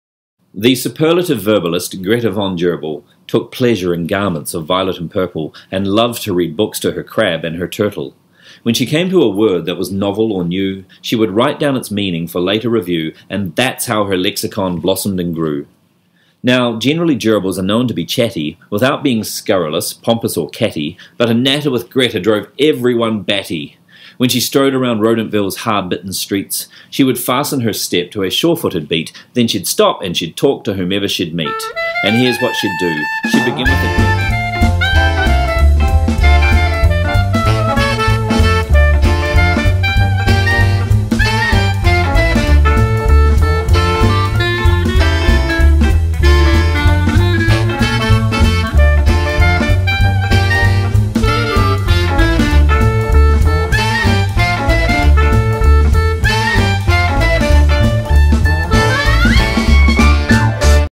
Greta von Gerbil Trailer 9D150AE7 D8A0 4E7F 9D27 CBEBDEAF1D57
Greta von Gerbil is a tiny little rodent with a really large vocabulary. When she walks around Rodentville using lots of big words, her fellow townsfolk feel threatened. A pair of Chipmunks decide to steal her thesaurus, but just before they can destroy it, the local gangsters think of a cunning plan. They hope to outsmart Greta by using her thesaurus to make sentences with the biggest words they can find, and a crazy word battle commences.

Greta von Gerbil is a comical narrative in verse that encourages vocabulary building and revels in the pleasures of language. For ages 10 and up.